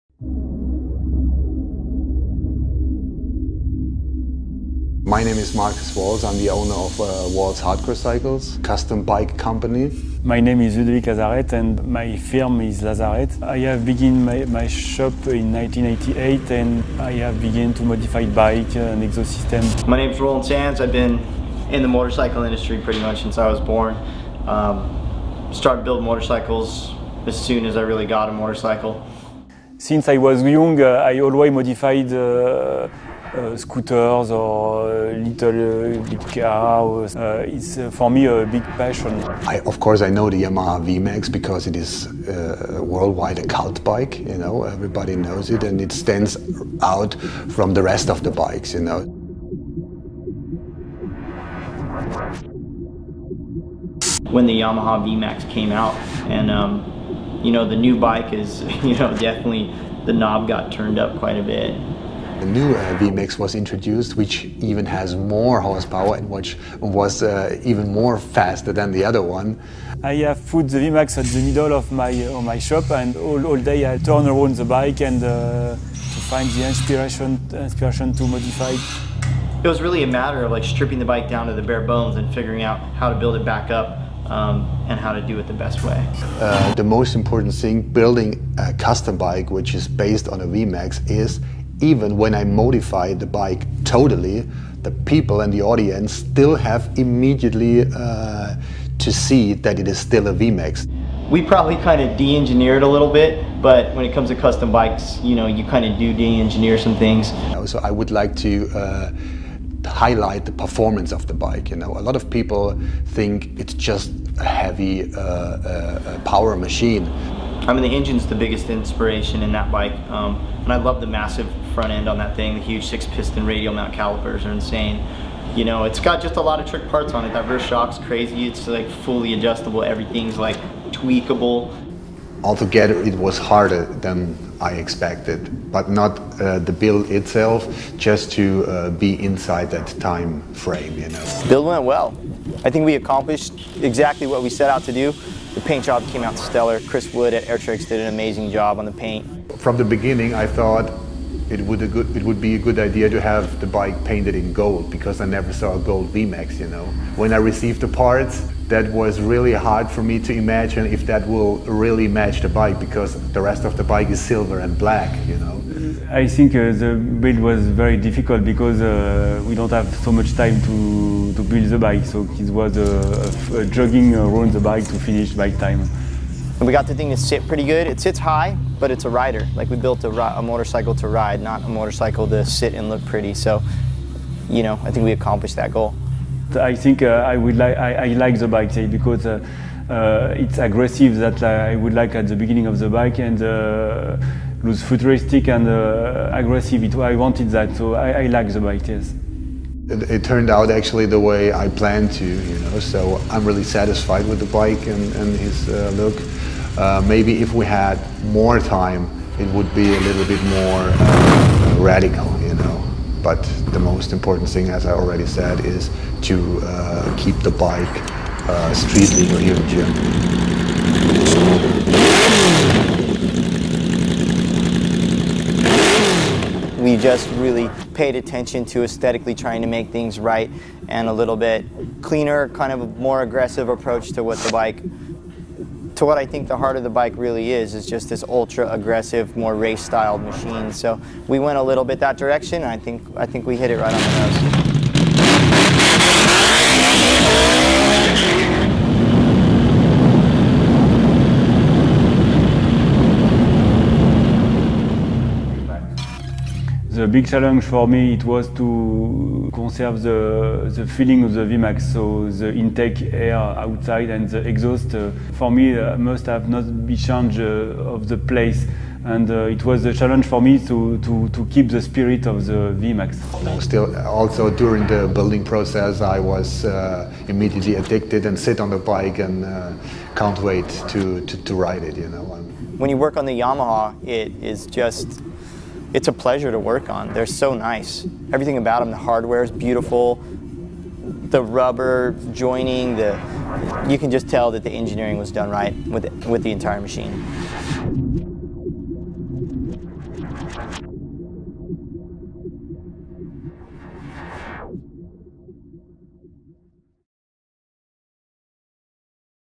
VMAX HyperModified Bikes introduction
Take the world's best biking brains, those with imagination, tools, mettle and metal to transform an iconic machine, the Yamaha VMAX, into something even more individual.

Ludovic Lazareth is one of the Custom Builders who took this unique opportunity to create a hyper-modified masterpiece.

Frenchman Ludovic has a factory in Annecy-le-Vieux, near the Swiss border, where he turns out two, three and four-wheel creations -- including unique car/bike hybrids such as R1-engined bespoke quads and a supercharged 84bhp Yamaha TMax.